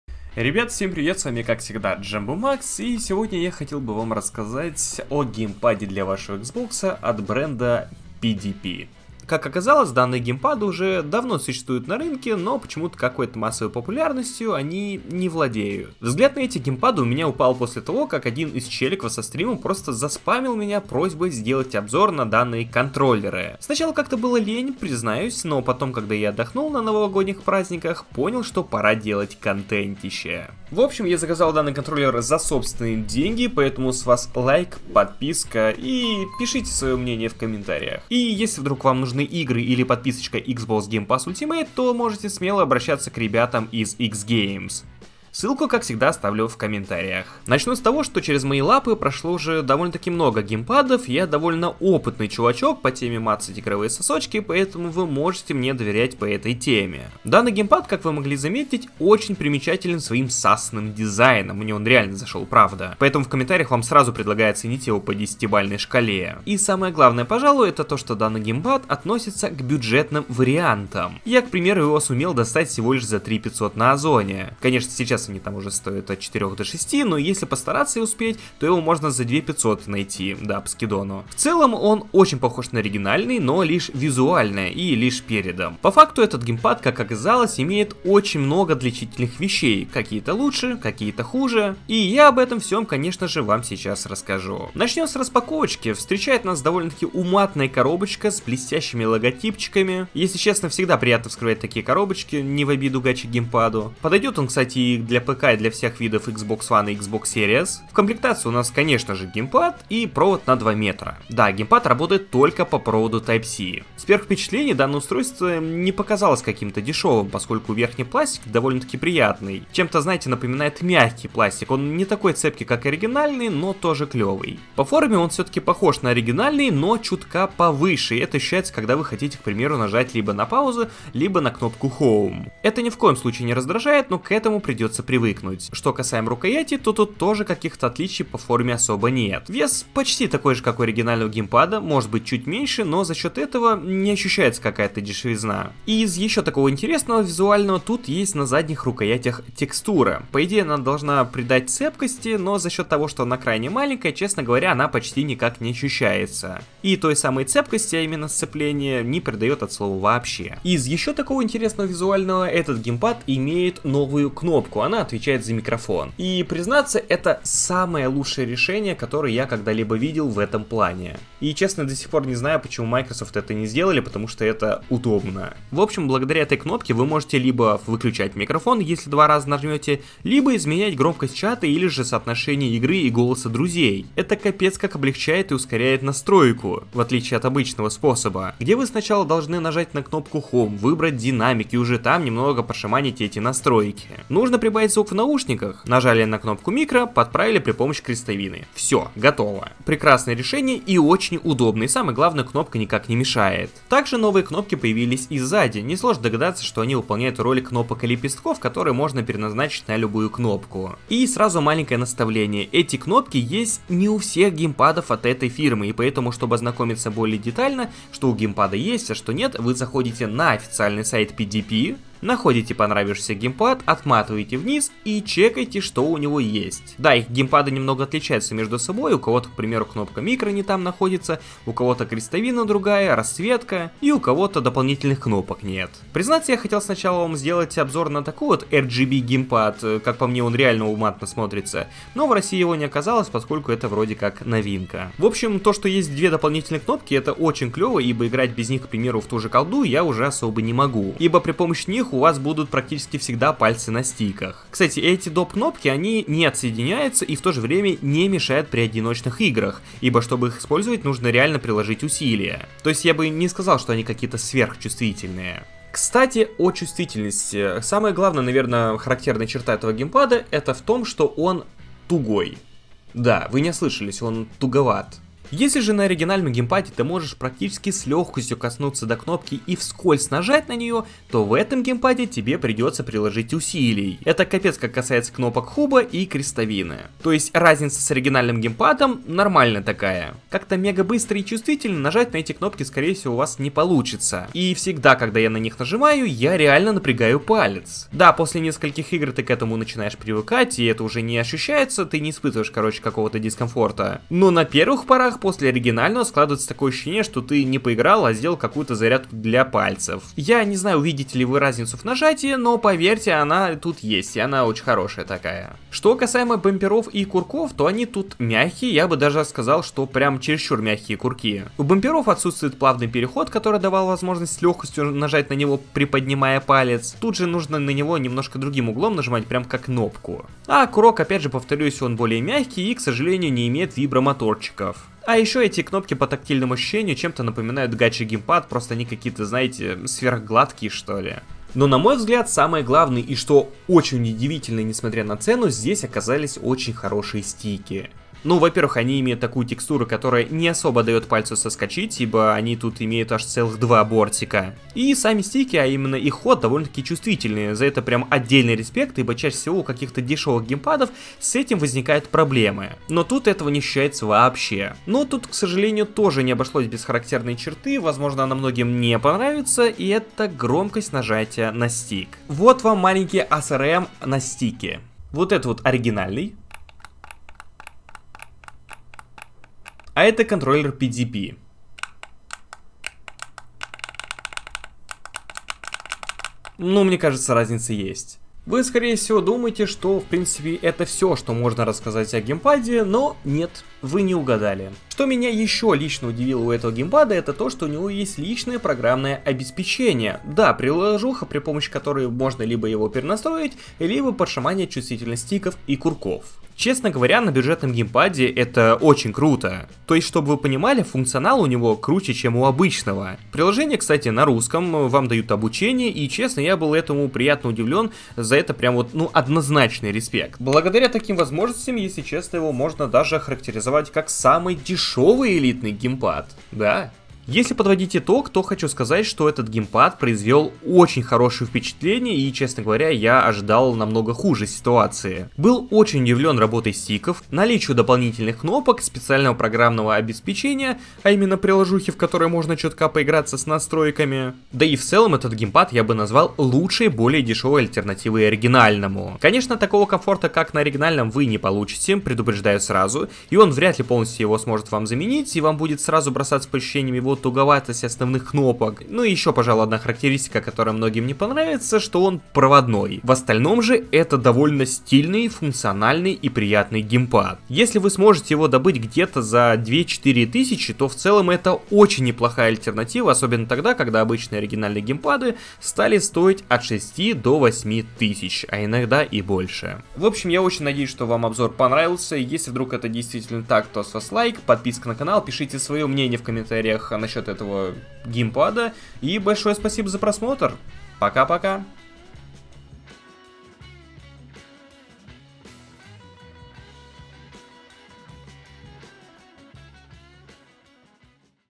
ЛУЧШИЙ ДЕШЁВЫЙ ГЕЙМПАД ДЛЯ XBOX // PDP Series X|S
Расскажу о новом бюджетном геймпаде Проводной геймпад PDP Glitch Green Rematch для Xbox Series\One\PC. Расскажу какие новые кнопки тут есть, затрону материал, стики, а так же расскажу о том, чем он отличается от обычного геймпада.